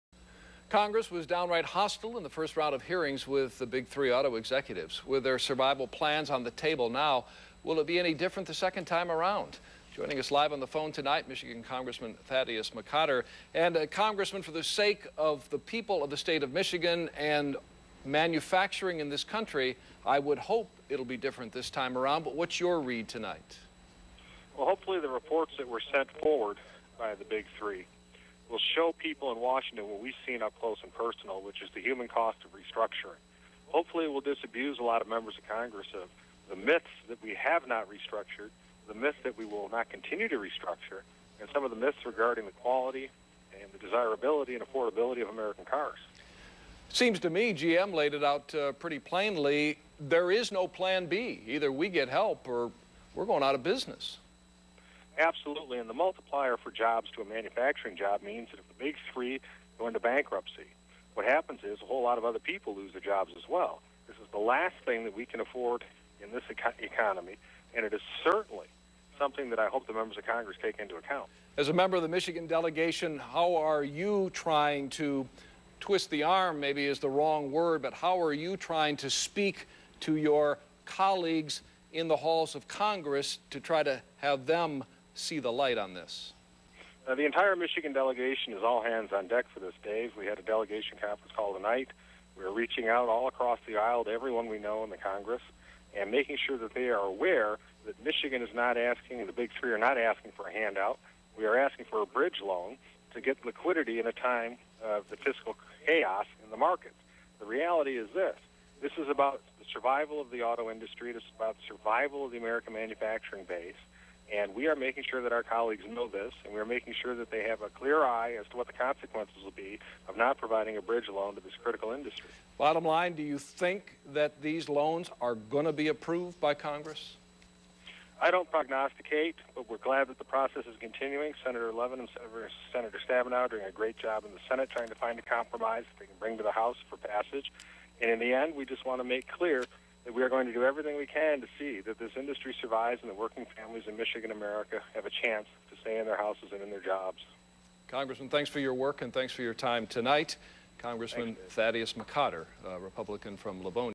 McCotter Praises Michigan Delegation for Auto Support on WXYZ 7 Detroit
Rep. Thaddeus McCotter urges help for automakers